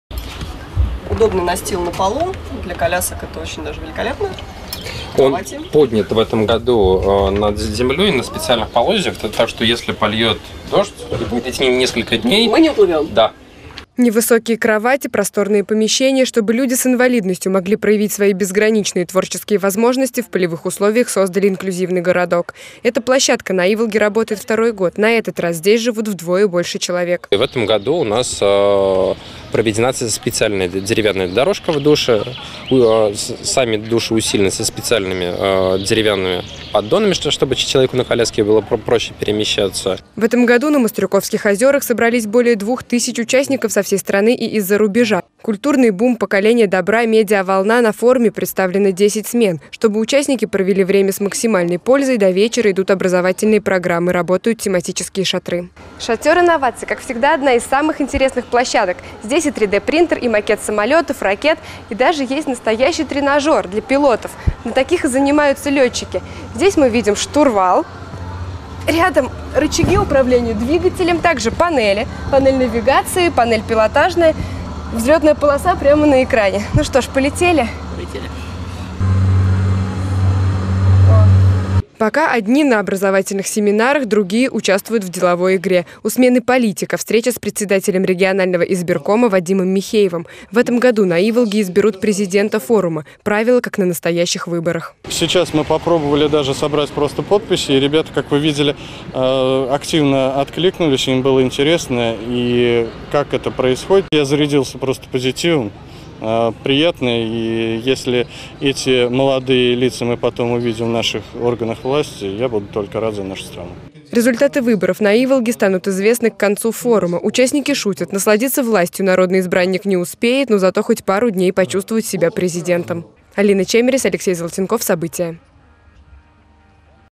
На молодежном форуме "iВолга" можно научиться управлять самолетом
В Самарской области на Мастрюковских озерах проходит молодежный форум "iВолга-2016". Образовательные программы, творческие мастер-классы, и, конечно, конвейер проектов. Участники получают ценные знания и возможность реализовать даже самые смелые амбиции. За 10 дней можно научиться водить самолет и даже стать президентом